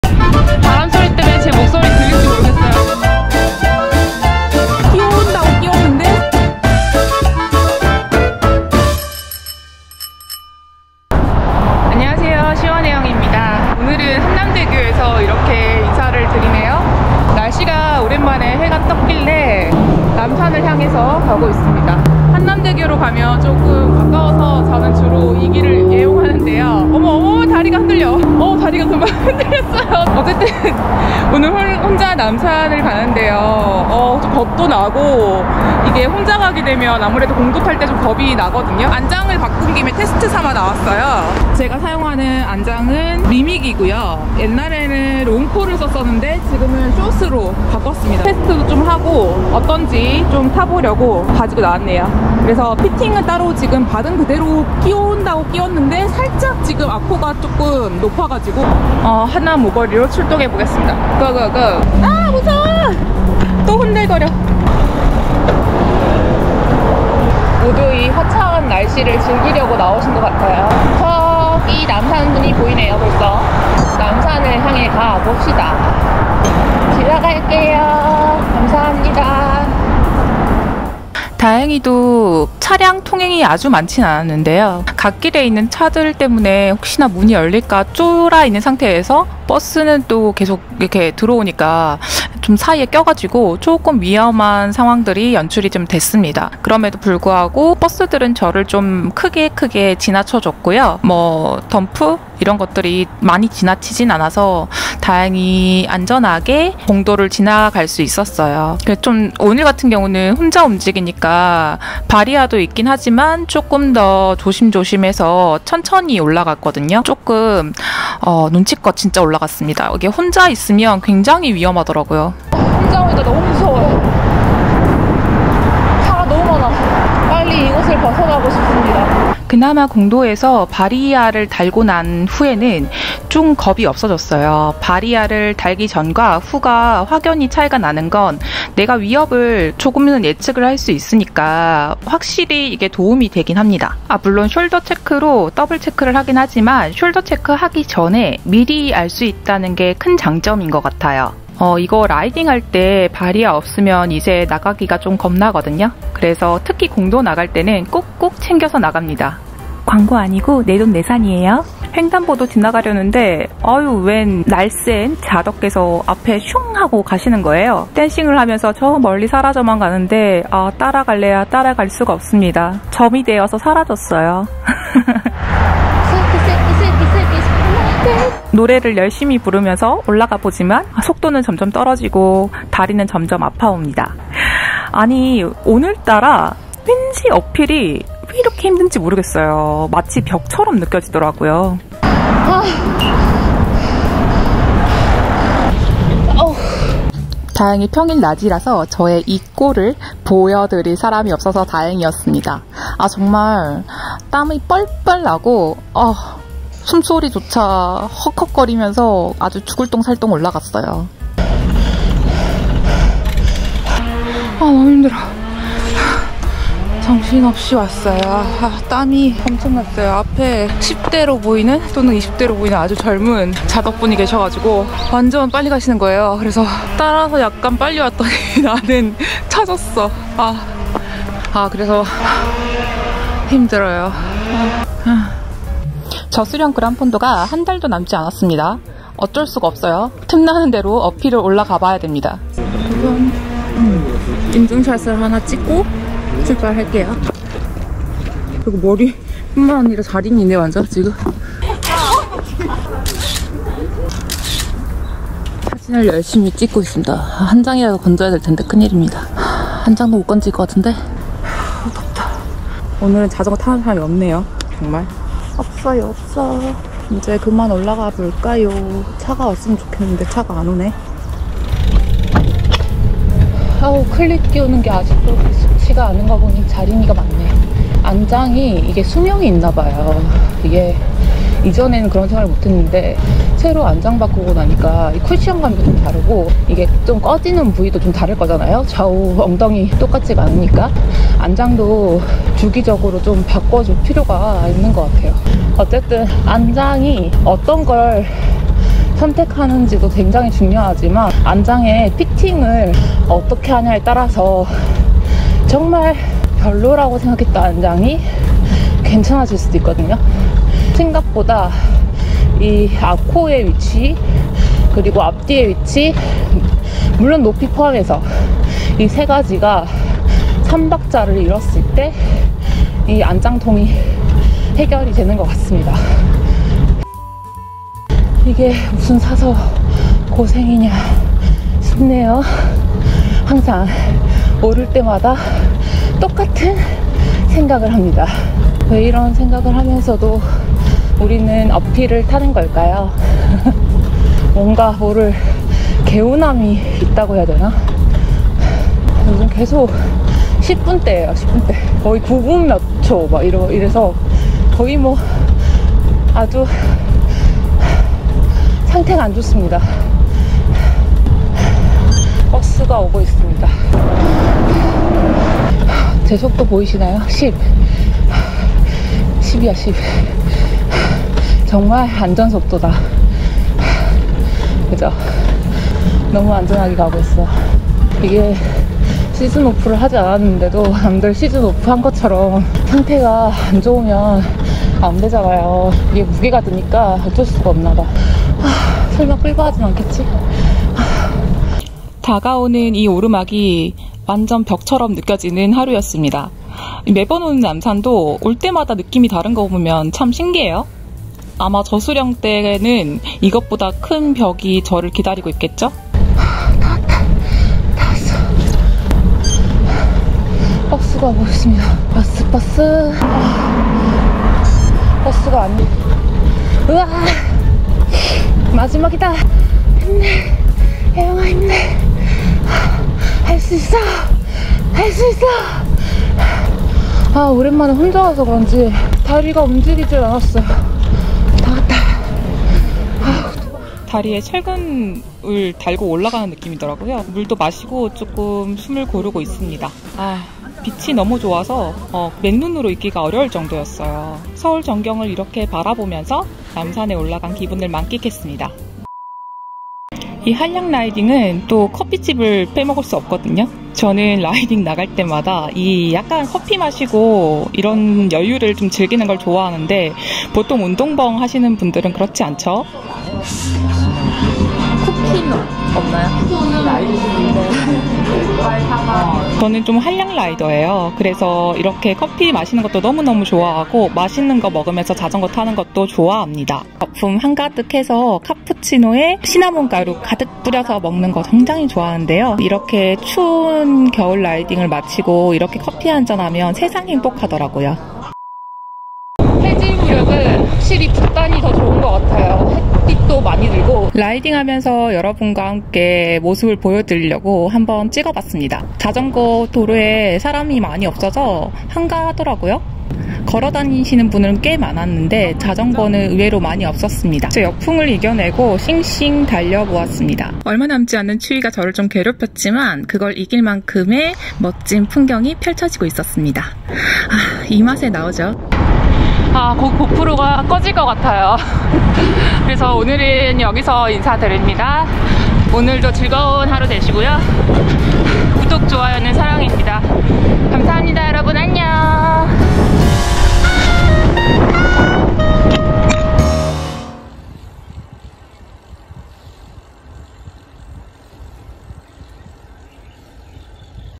🛍️난 백화점 대신 남산간다 ⎮남산솔라
안장 테스트+ 저수령 그란폰도 준비 겸￼ 남산 라이딩을 다녀왔습니다
혼자가서 겁도 났는데 … 과연 오늘은 무슨 일이 있었을까요?

응원과 따뜻한 댓글 항상 감사합니다.

무단 편집, 업로드 및 재배포를 엄격히 금지합니다.